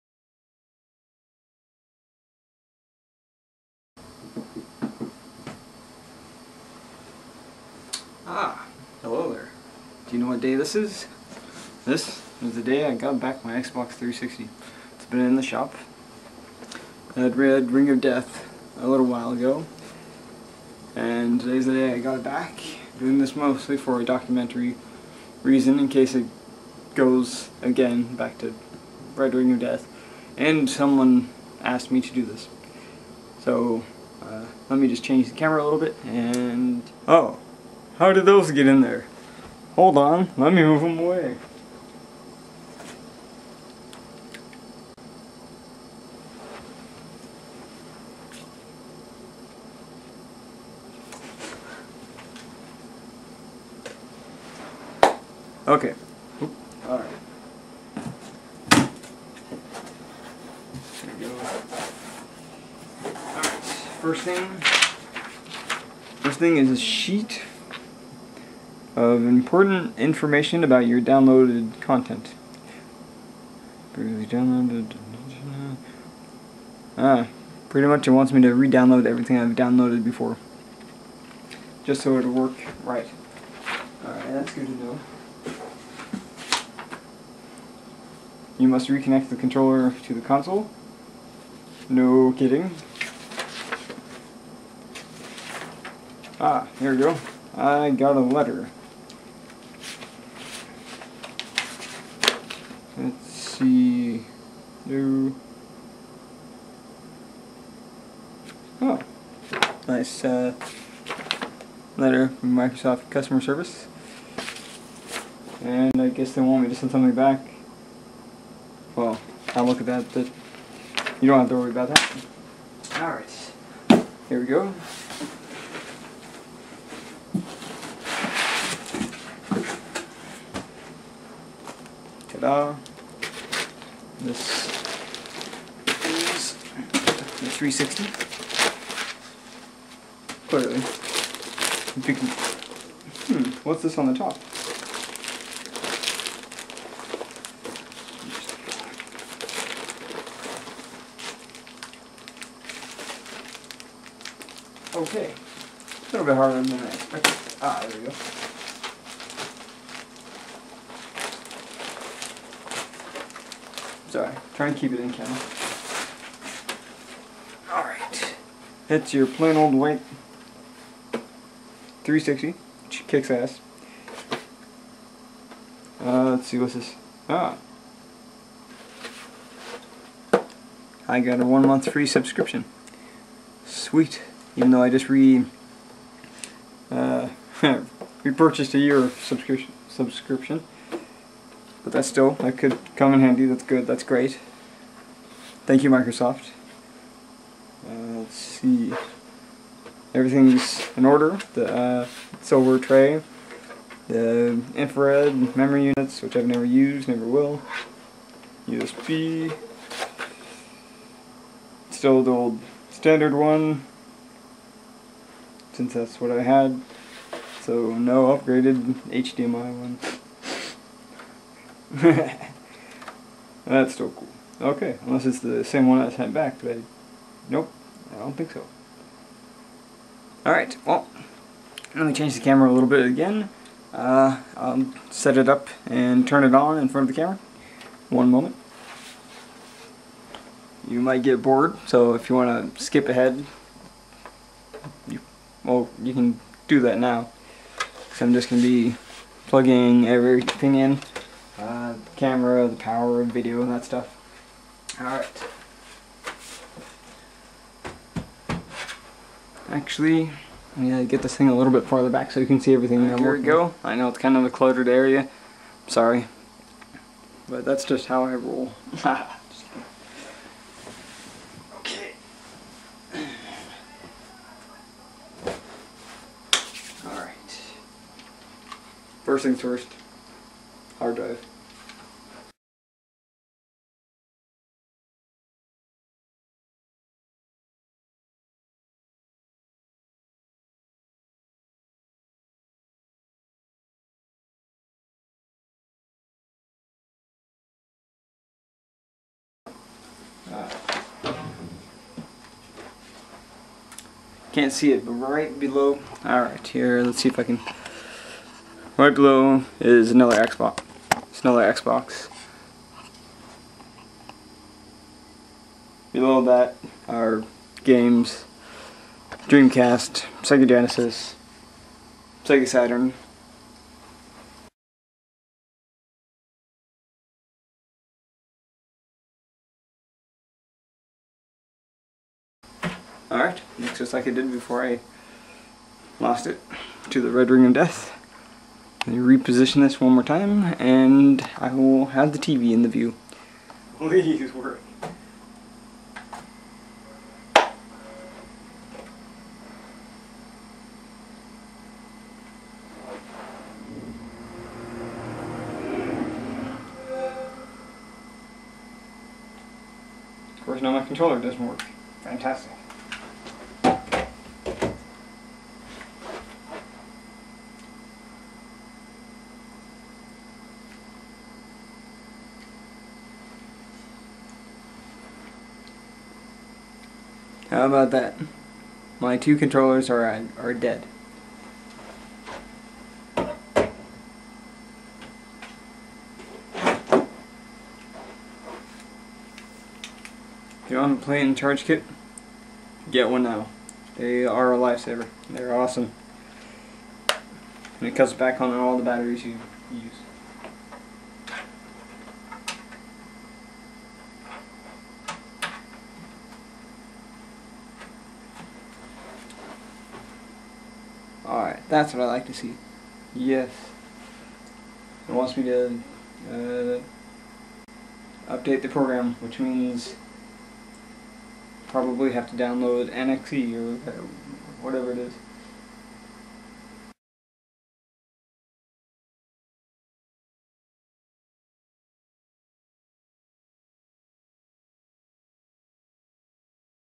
Xbox 360 is back
Red ring of death no more, my 360 is back